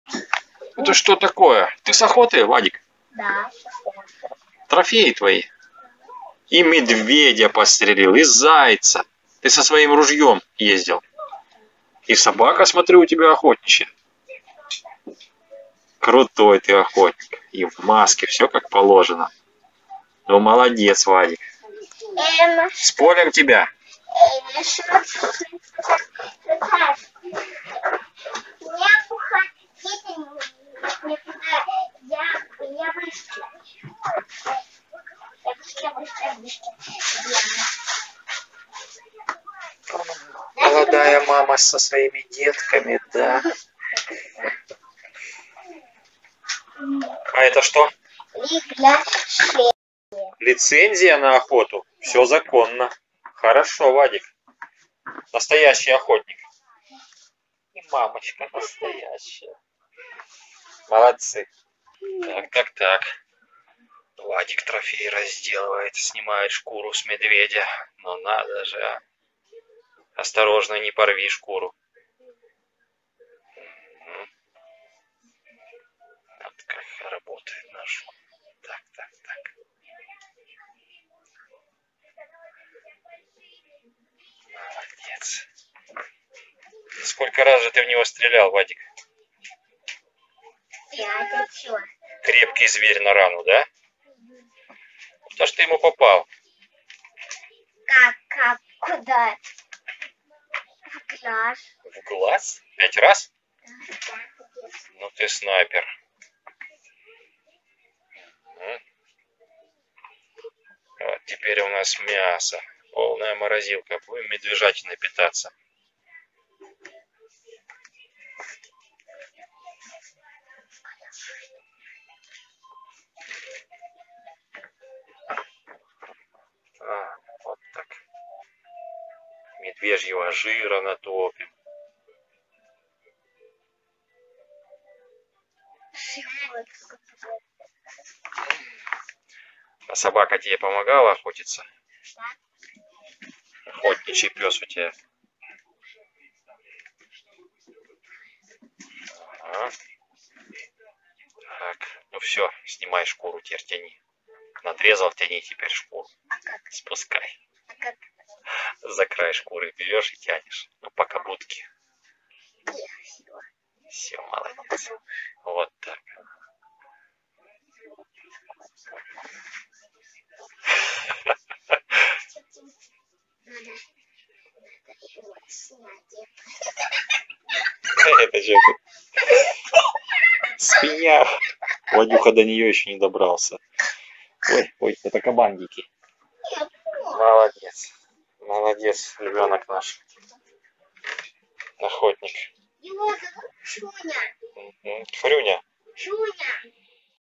Разделка медведя!Вадим приехал с охоты!С трофеями,всё законно!
Инспектор может спать спокойно!
У Вадима всё законно!
Зайцам тоже нечего боятся
После Ирбис есть кому остаться!
Купить отличные ножи для охоты и не только:
На форуме